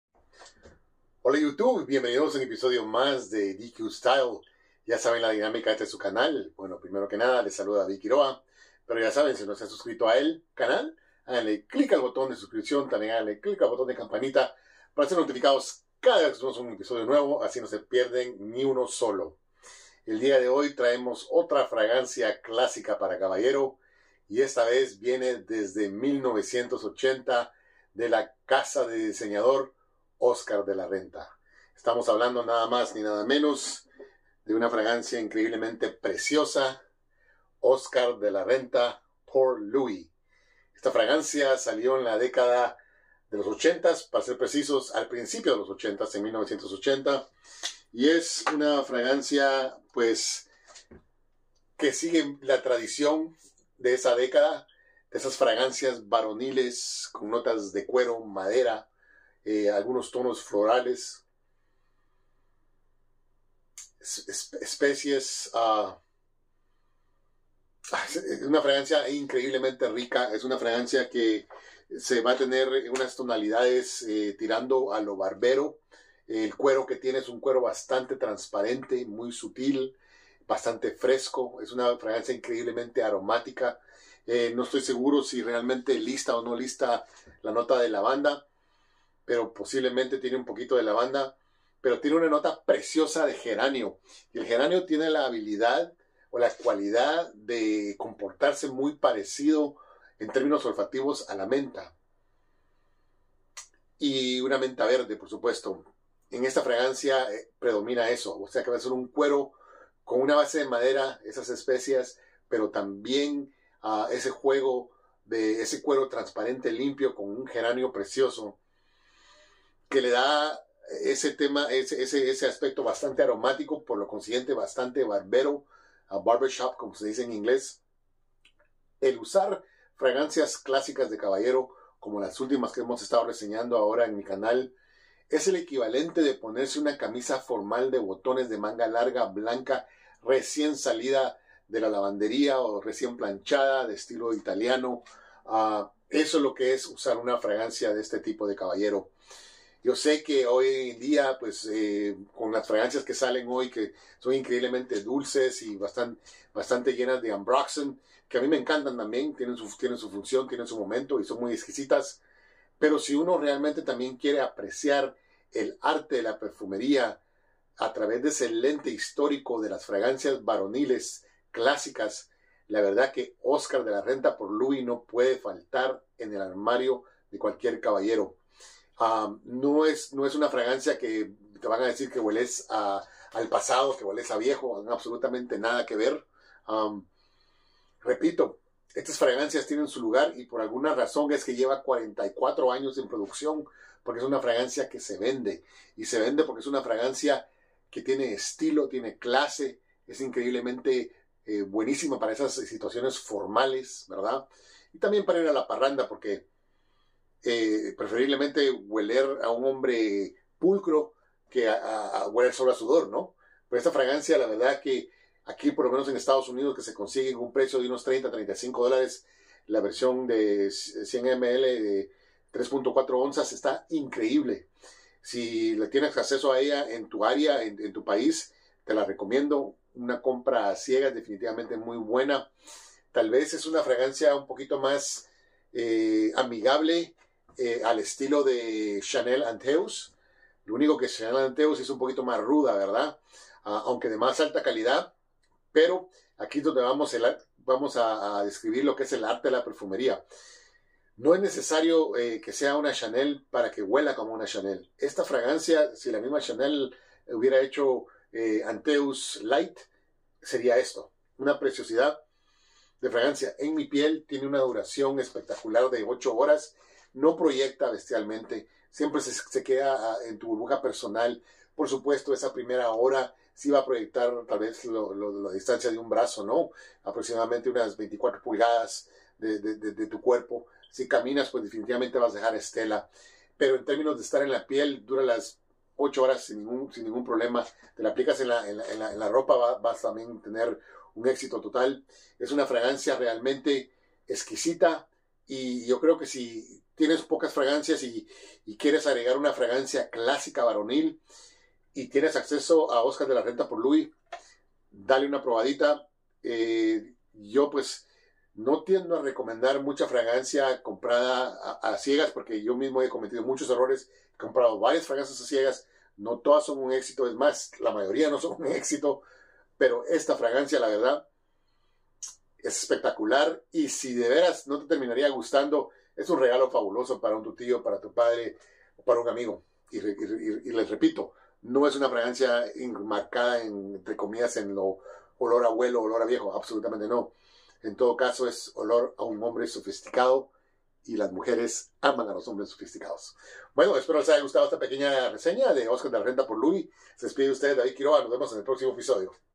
Oscar de la Renta Pour Lui. Una fragancia para un hombre de exito.
Una fragancia formal para hombre de exito. Oscar de la Renta Pour Lui